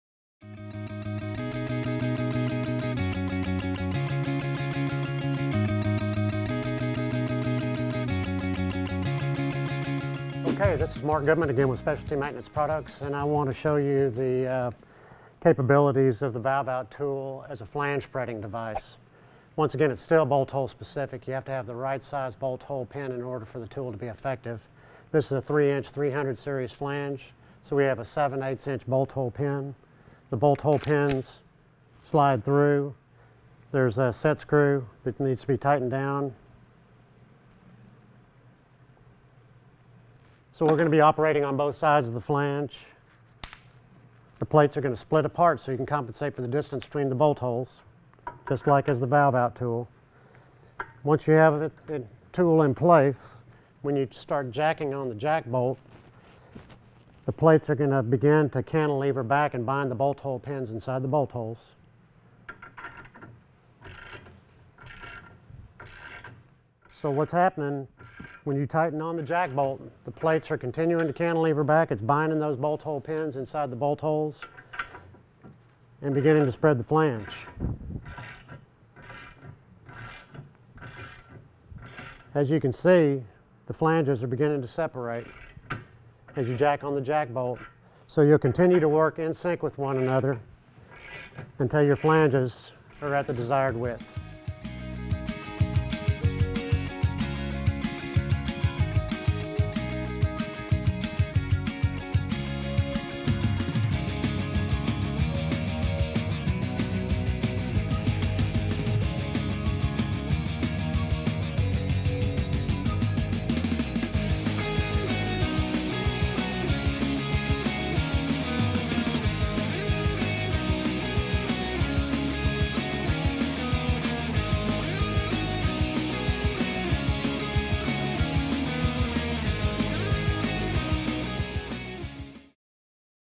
SMP Valve Out Tool as a Flange Spreader Demonstration
*** Tool Description ***

SMP’s Valve-Out Tool is a bolt-hole specific tool. With the Valve-Out Tool valve spreader you can eliminate the need for come-alongs, track-hoes, wire rope, chains, hammers and wedges.
When you turn the jack-bolt, the plates cantilever back, binding the bolt-hole pins in the bolt-holes, pulling the outside flanges back, spreading the flanges up to 1/2″ The SMP Valve-Out flange spreader tool requires no gap and is an excellent tool for changing gaskets.
In addition, SMP’s Valve-Out Tool valve spreader allows for safe and easy removal of control valves, by-pass valves, expansion joints, spools, rupture disks, regulators, orifice plates, and meters.
The SMP VOC1 Kit – 4 spreading plates, 2 sets (11.5” each) of spacer bar segments, and 8 each interchangeable bolt hole pins 3/4″, 7/8”, and 1”
The SMP VOC2 Kit – 4 spreading plates, 2 sets (11.5” each) of spacer bar segments, and 8 each interchangeable bolt hole pins 1-1/8”, 1-1/4”, and 1-3/8”
The SMP VOC1-2 Kit – 4 spreading plates, 2 sets (11.5” each) of spacer bar segments, and 8 each interchangeable bolt hole pins 3/4″, 7/8”, 1”, 1-1/8”, 1-1/4”, and 1-3/8”
All components are contained in hard shell carrying cases with custom foam inserts.

Extra Spacer Bars Available for Valves Wider than 14″
VOC SPCR2 – 2″ Valve-Out Spacer Bar
VOC SPCR 4 – 4″ Valve-Out Spacer Bar
VOC SPCR6 – 6″ Valve-Out Spacer Bar

Check out this tool and more on our websites!